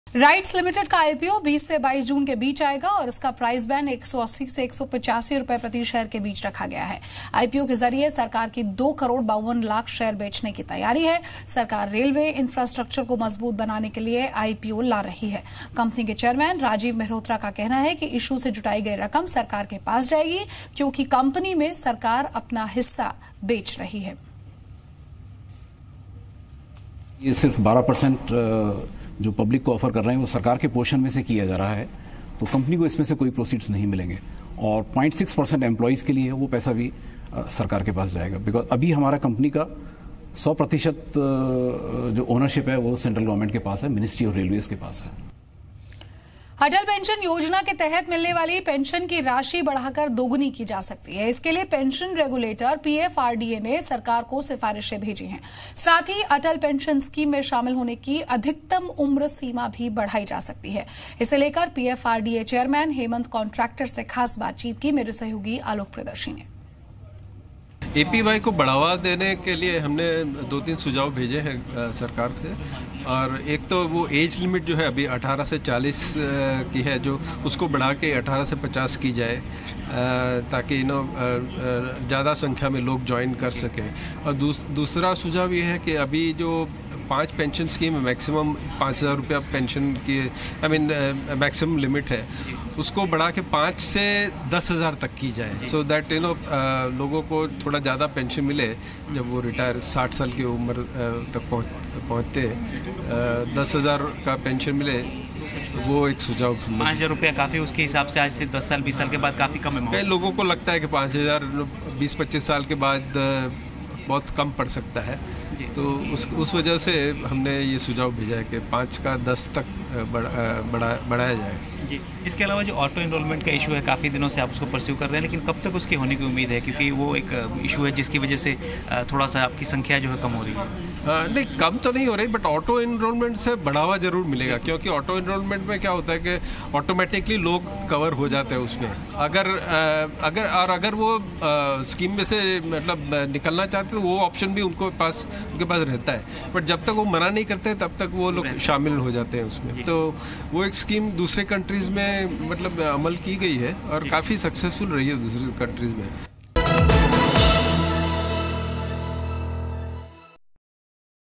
IPO of RITES Limited to be launched between June 20-22
This is a segment of Zee Business which talks about IPO of RITES Limited that will be launched between June 20-22. Watch this video for more information.